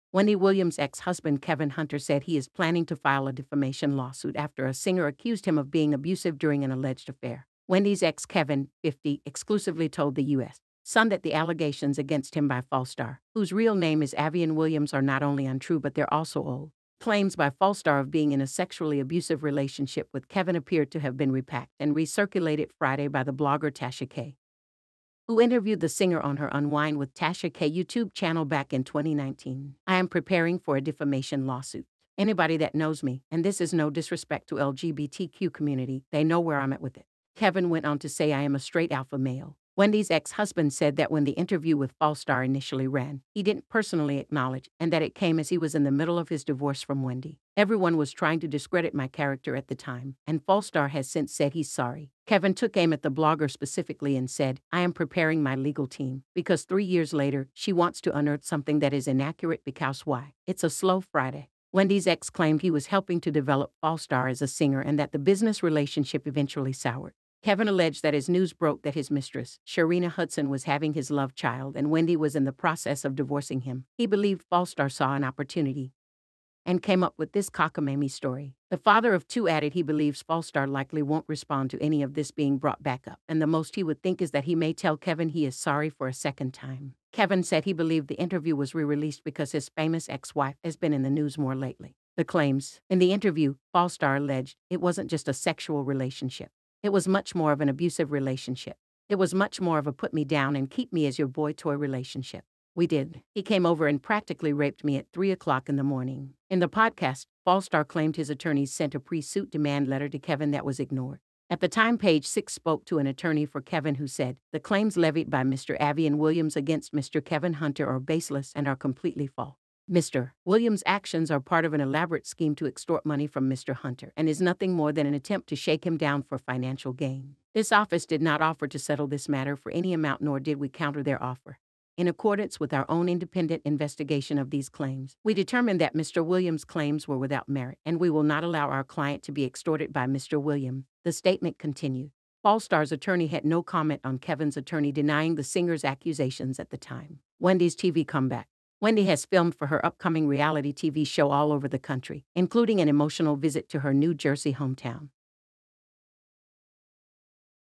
Wendy Williams’ ex-husband Kevin Hunter to file lawsuit after male singer accused him
WENDY Williams' ex-husband Kevin Hunter said he is planning to file a defamation lawsuit after a singer accused him of being abusive during an alleged affair.

Wendy's ex Kevin, 50, exclusively told The U.S. Sun that the allegations against him by Falstar, whose real name is Aveon Williams are not only untrue but they're also old.

Claims by Falstar of being in a sexually abusive relationship with Kevin appeared to have been repacked and re-circulated Friday by the blogger Tasha K, who interviewed the singer on her Unwine With Tasha K YouTube channel back in 2019.

"I am preparing for a defamation lawsuit. Anybody that knows me, and this is no disrespect to LGBTQ community, they know where I'm at with it."

Kevin went on to say "I am a straight alpha male."

Wendy's ex-husband said that when the interview with Falstar initially ran, he didn't personally acknowledge it and that it came as he was in the middle of his divorce from Wendy.

"Everyone was trying to discredit my character at the time, and Falstar has since said he's sorry. "

Kevin took aim at the blogger specifically and said: "I am preparing my legal team, because three years later, she wants to unearth something that is inaccurate becaus why, its a slow Friday?"

Wendy's ex claimed he was helping to develop Falstar as a singer and that the business relationship eventually soured.

Kevin alleged that as news broke that his mistress, Sharina Hudson was having his love child and Wendy was in the process of divorcing him, he believed Falstar saw an opportunity and came up with this "cockamamie story."

The father-of-two added he believes Falstar likely won't respond to any of this being brought back up and the most he would think is that he may tell Kevin he is sorry for a second time.

Kevin said he believed the interview was re-released because his famous ex-wife has been in the news more lately.

THE CLAIMS
In the interview, Falstar alleged: “It wasn’t just a sexual relationship. It was much more of an abusive relationship."

“It was much more of a put me down and keep me as your boy toy relationship. We did [have sex] … He came over and practically raped me at 3 o’clock in the morning.”

In the podcast, Falstar claimed his attorneys sent a “pre-suit” demand letter to Kevin that was ignored.

At the time PageSix spoke to an attorney for Kevin who said: “The claims levied by Mr. Aveon Williams against Mr. Kevin Hunter are baseless and are completely false.

“Mr. Williams’ actions are part of an elaborate scheme to extort money from Mr. Hunter and is nothing more than an attempt to shake him down for financial gain.”

“This office did not offer to settle this matter for any amount nor did we counter their offer as, in accordance with our own independent investigation of these claims, we determined that Mr. Williams’ claims were without merit and we will not allow our client to be extorted by Mr. Williams,” the statement continued.

Falstar’s attorney had no comment on Kevin's attorney denying the singer's accusations at the time.

WENDY'S TV COMEBACK
Wendy has filmed for her upcoming reality TV show all over the country, including an emotional visit to her New Jersey hometown.

The U.S. Sun can recently revealed that members of Wendy's filming crew let slip that the show is set to be released this fall.

The ex-TV host's reality show style project was first exclusively revealed by The U.S. Sun after she was spotted with a small film crew at her favorite restaurant Fresco by Scotto in midtown.